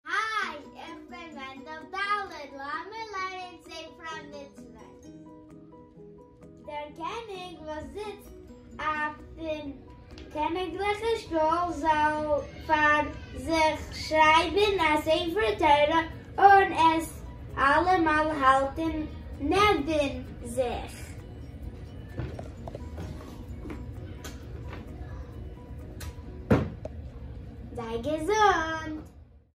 Sefer Hamitzvos for Kids Yiddish Shiur 29
בס״ד

ג׳ מנחם אב תשפ״א

לערן ספר המצוות מיט מענדל ד! אין דורכשניט פון אומגעפער איין יאר דו וועסט לערנען אלע מצוות פון אונזער הייליקע תורה לויטן רמב"ם. באטייליג זיך מיט מיר און זע ווי אינטערעסאנט אידישקייט קענען זיין.